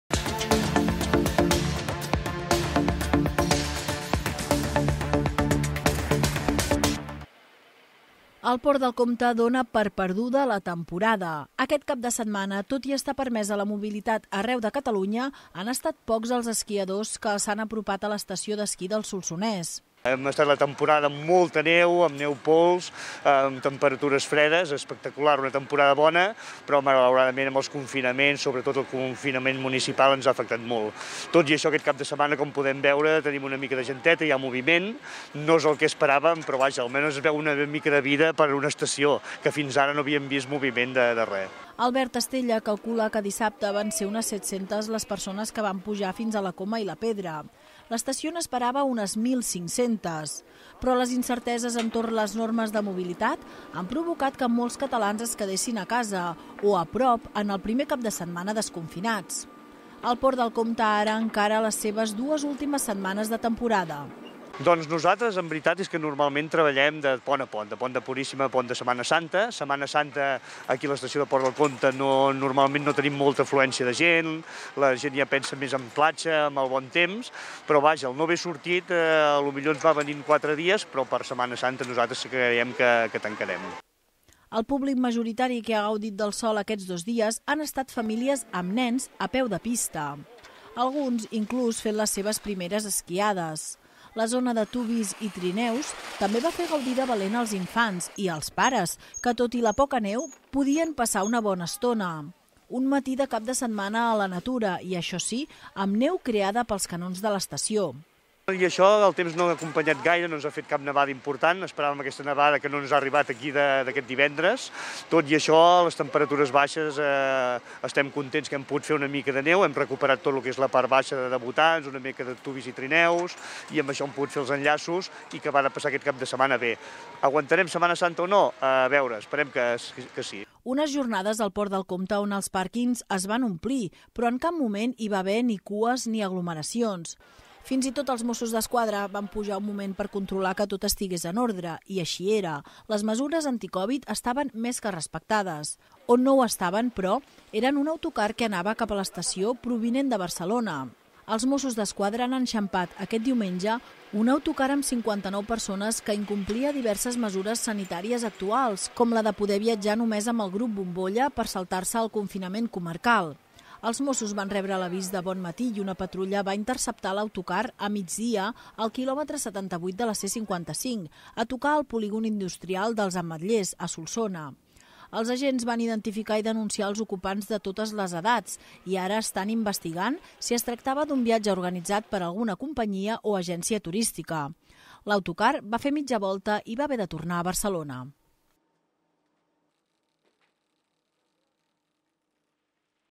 El Port del Comte dona per perduda la temporada
El Port del Comte dona per perduda la temporada. Aquest cap de setmana, tot i estar permesa la mobilitat arreu de Catalunya, han estat pocs esquiadors els que s’han apropat a l’estació d’esquí del Solsonès.

Albert Estella calcula que dissabte van ser unes 700 les persones que van pujar fins La Coma i la Pedra. L’estació n’esperava unes 1.500. Però les incerteses entorn les normes de mobilitat han provocat que molts catalans es quedessin a casa o a prop en el primer cap de setmana desconfinats. El Port del Comte ara encara les seves dues últimes setmanes de temporada.
El públic majoritari que ha gaudit del sol d’aquests dos dies han estat famílies amb nens que a peu de pista. Alguns, inclús, fent les seves primeres esquiades. La zona de tubbies i trineus també va fer gaudir de valent als infants i als pares que, tot i la poc neu, podien passar una bona estona. Un matí de cap de setmana a la natura i, això sí, amb neu creada pels canons de l’estació.
Unes jornades al Port del Comte on els pàrquings es van omplir però en cap moment hi va haver ni cues ni aglomeracions. Fins i tot, els Mossos d’esquadra van pujar un moment per controlar que tot estigués en ordre i així era, les mesures anticovid estaven més que respectades. On no ho estaven però era en un autocar que anava cap a l’estació provinent de Barcelona. Els Mossos d'Esquadra han enxampat aquest diumenge un autocar amb 59 persones que incomplia diverses mesures sanitàries actuals, com la de poder viatjar només amb el grup bombolla per saltar-se el confinament comarcal. Els Mossos han rebut l'avís de bon matí i una patrulla ha interceptat l'autocar a migdia al quilòmetre 78 de la C-55, a tocar del polígon industrial dels Ametllers a Solsona. Els agents han identificat i denunciat els ocupants de totes les edats i ara estan investigant si es tractava d'un viatge organitzat per alguna companyia o agència turística. L'autocar ha fet mitja volta i ha hagut de tornar a Barcelona.